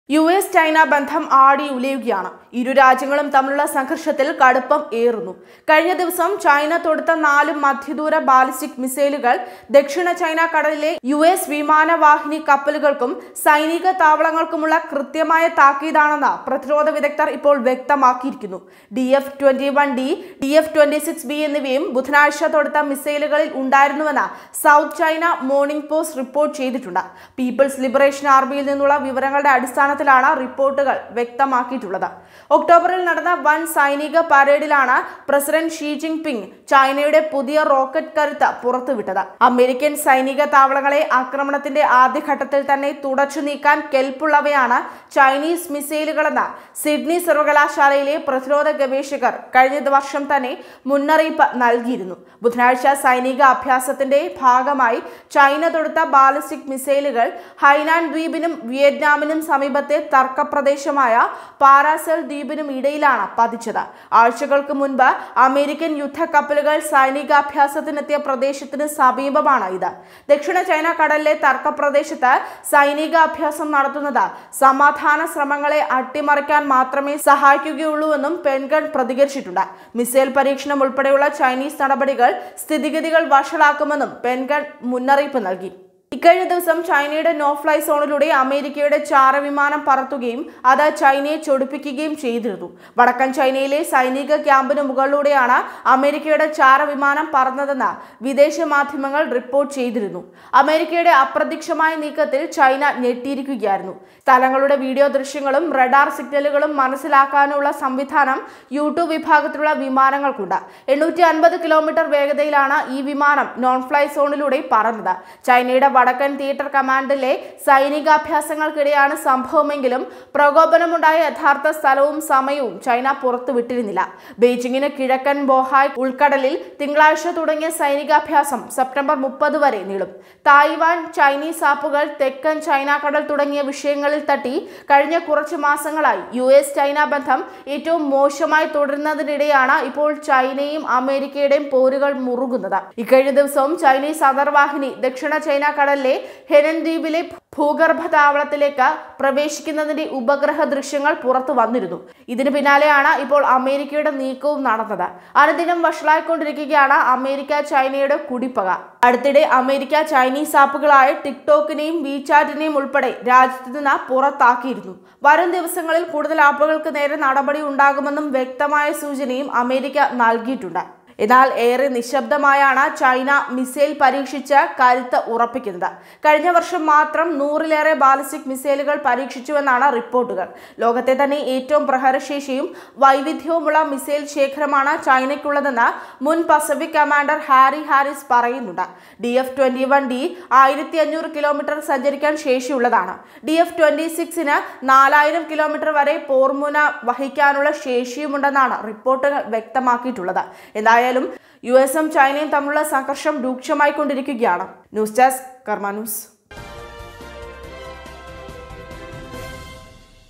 അനുദിനം വഷളായി അമേരിക്ക-ചൈന കുടിപ്പക.
യുഎസ് വിമാനവാഹിനി കപ്പലുകള്‍ക്കും സൈനിക താവളങ്ങള്‍ക്കും താക്കീതു നൽകി ചൈന,